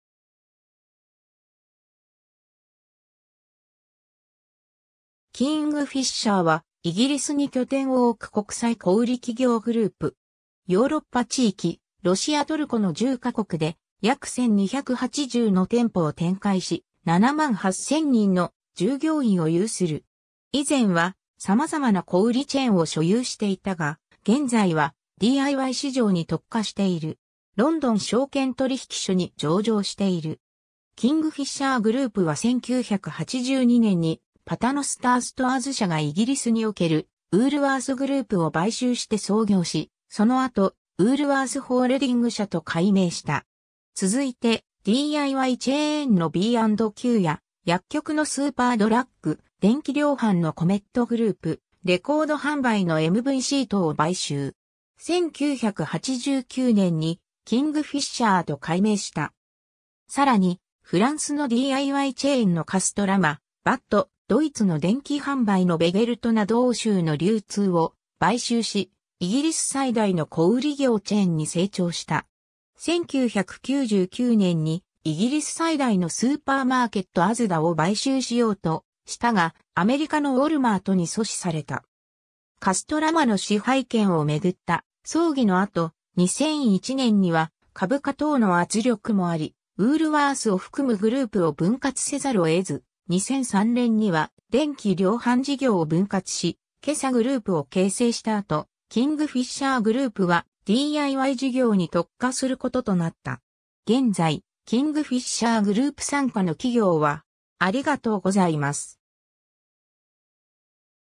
キングフィッシャー (小売業)
キングフィッシャー (英: Kingfisher plc) は、イギリスに拠点を置く国際小売企業グループ。
ヨーロッパ地域・ロシア・トルコの10カ国で約1,280の店舗を展開し、78,000人の従業員を有する。
以前は様々な小売りチェーンを所有していたが、現在はDIY市場に特化している。
ロンドン証券取引所に上場している。
 キングフィッシャー・グループは1982年にパタノスター・ストアーズ社（Paternoster Stores Ltd）がイギリスにおけるウールワース・グループを買収して創業し、その後ウールワース・ホールディング社（Woolworths Holdings plc）と改名した。
続いてDIYチェーンのB&Qや薬局のスーパードラッグ、電器量販のコメット・グループ（Comet Group plc）、レコード販売のMVC（Music and Video Club）等を買収。
1989年にキングフィッシャー（Kingfisher plc）と改名した。
 さらにフランスのDIYチェーンのカストラマ（Castorama）、BUT、ドイツの電器販売のヴェゲルト（Wegert）など欧州の流通を買収し、イギリス最大の小売業チェーンに成長した。
1999年にイギリス最大のスーパーマーケットASDAを買収しようとしたが、アメリカのウォルマートに阻止された。
カストラマの支配権を巡った争議の後、2001年には株価等の圧力もあり、ウールワースを含むグループを分割せざるを得ず、2003年には電器量販事業を分割し「KESA」グループを形成した後、キングフィッシャー・グループはDIY事業に特化することとなった。
 現在、キングフィッシャー・グループ傘下の企業は：